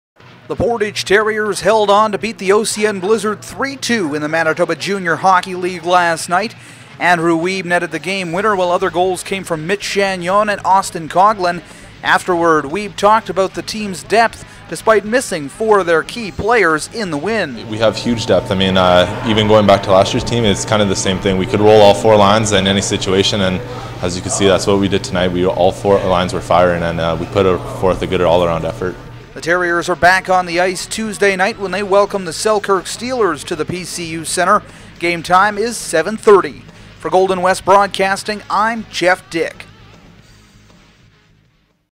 Terriers Hold On To Beat Blizzard
The Terriers got a pair of goals in the third to topple OCN 3-2 at the PCU Centre.